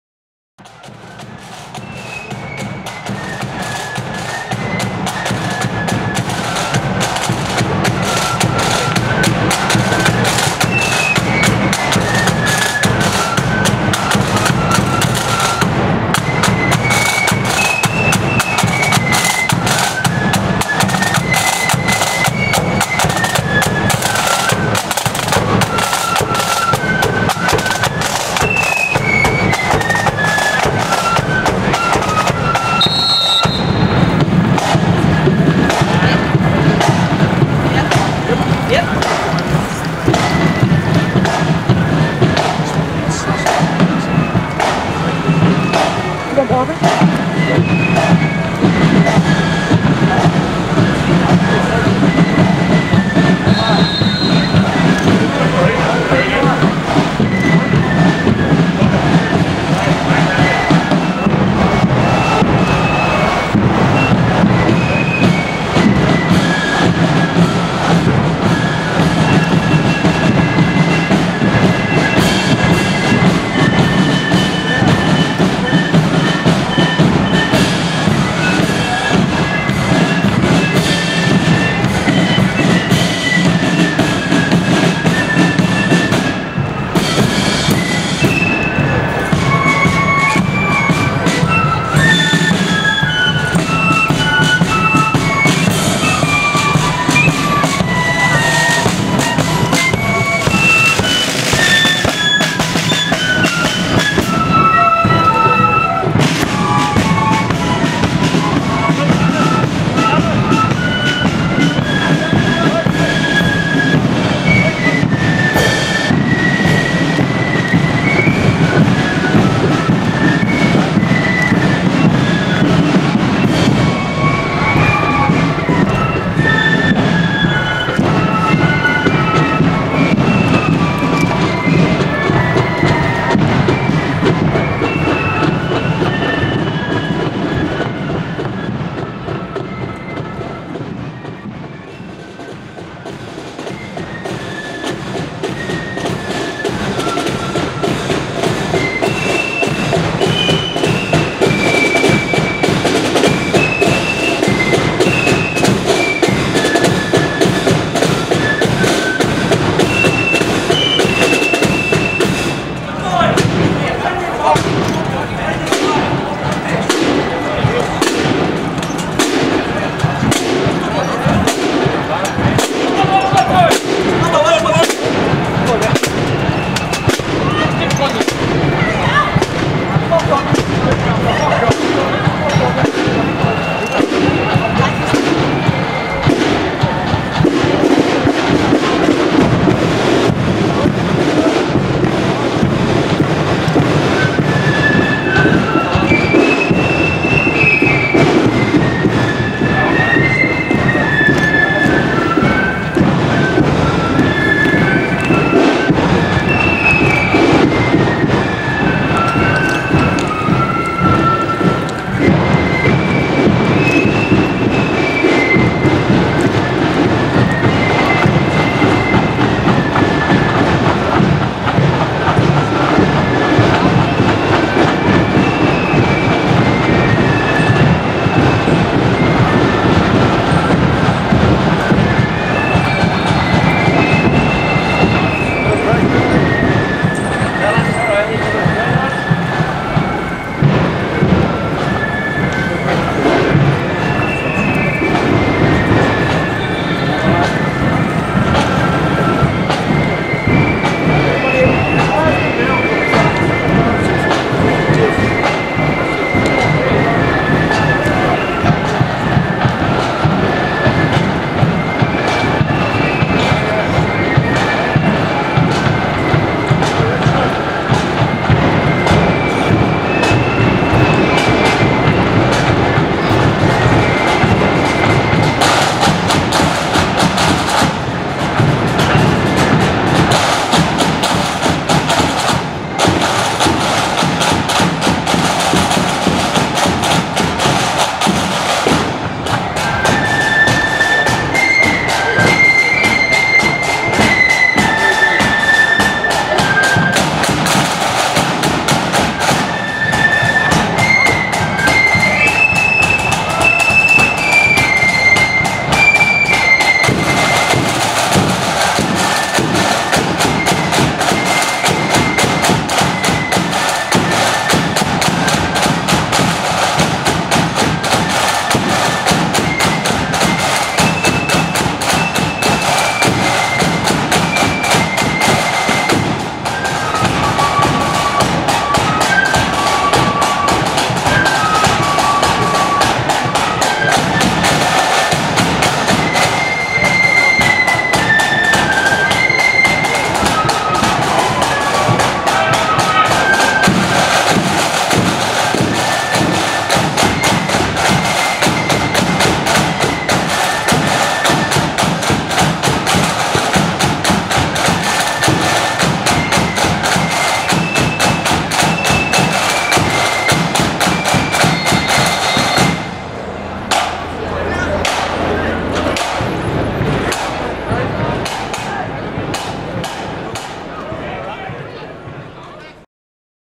36th Ulster Division Mem Association Parade Belfast 9/5/2015 (Part2)
36th Ulster Division Memorial Association parade to mark the 100th anniversary of the troops marching pass City hall to go to war.